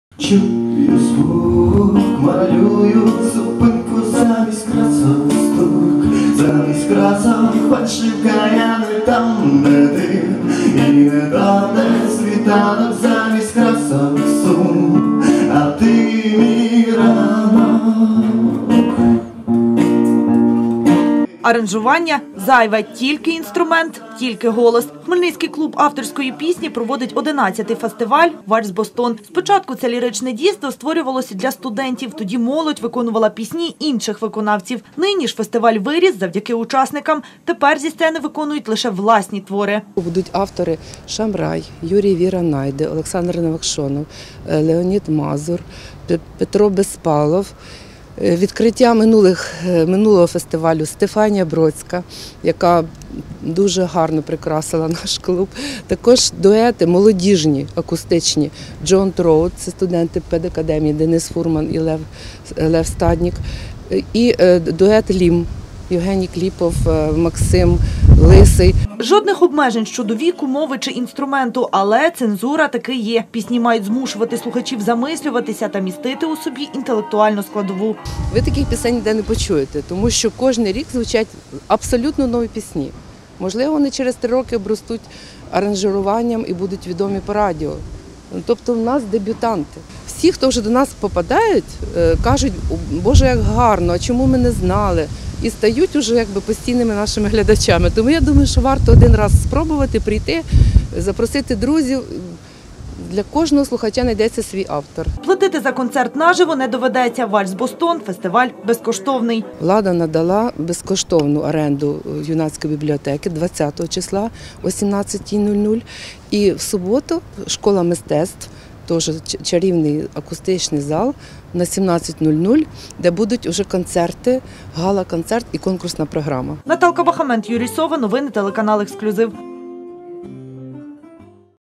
Хмельничан запрошують на «Вальс-бостон» - фестиваль авторської пісні
Осінь романтична. Хмельничан запрошують на «Вальс-бостон». Вже вдесяте, у нашому обласному центрі проходитиме фестиваль авторської пісні. Відбуватиметься фест протягом двох днів - у наступні четвер і суботу. Невідомі, але цікаві та оригінальні виконавці виконуватимуть власні композиції у супроводі акустики.